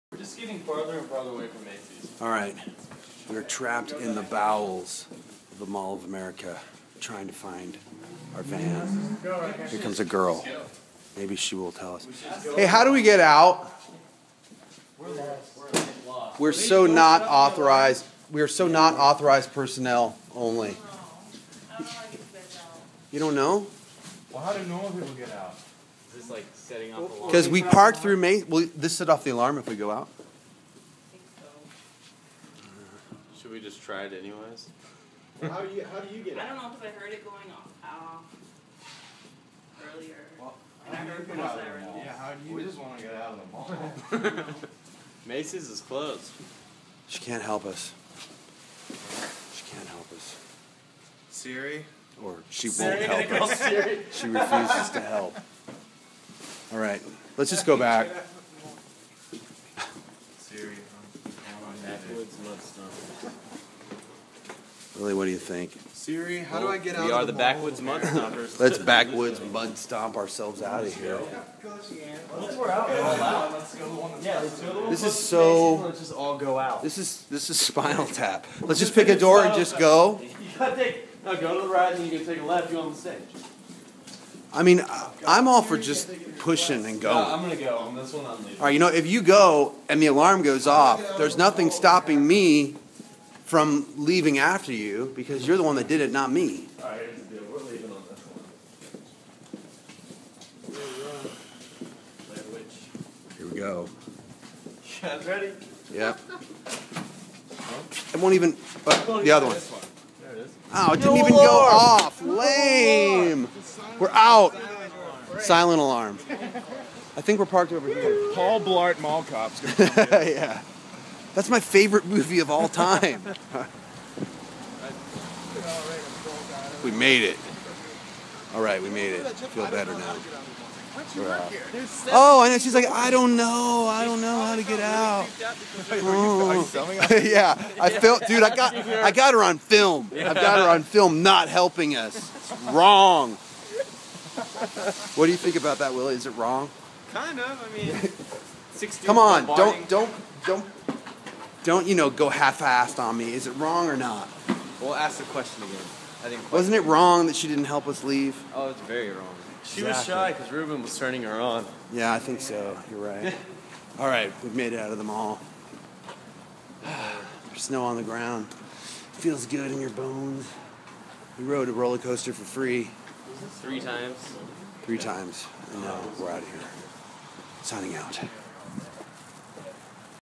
Trapped in Mall of America!
We parked outside of Macy's and Macy's was CLOSED!!! What else were we to do but go through an 'Authorized Personnel Only' door to try and get out?